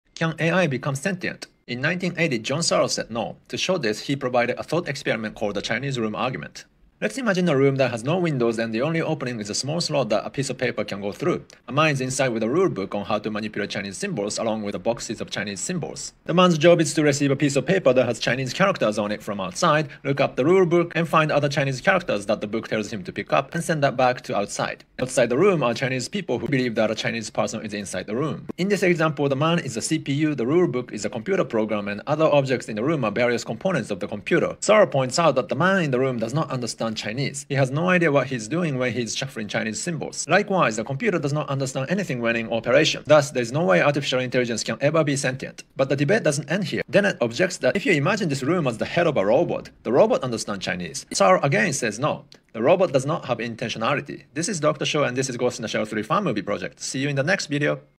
Chinese Room argument in Artificial intelligence Ghost in the Shell 3 fan movie project #015
John Searle's famous Chinese Room argument points out the impossibility of artificial intelligence achieving the status of sentient. Here is how the argument goes!
Here I'm documenting my scriptwriting for the Ghost in the Shell 3 fan movie project.